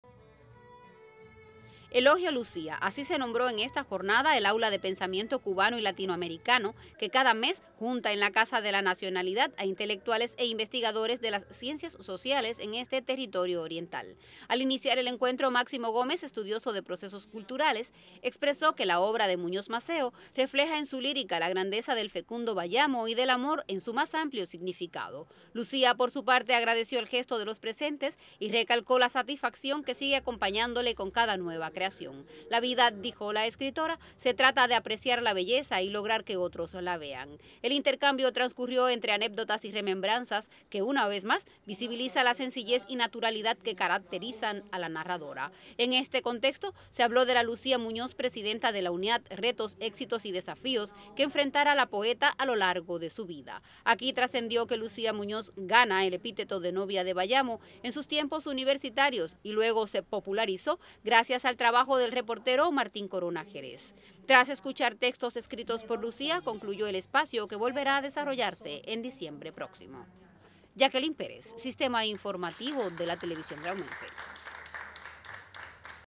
Rinden homenaje a Lucía Muñoz
La poetisa granmense Lucía Muñoz fue homenajeada en el espacio  Aula de Pensamiento Cubano y Latinoamericano. La cita de reflexión y debate tiene lugar cada mes bajo el auspicio de la Casa de la Nacionalidad Cubana.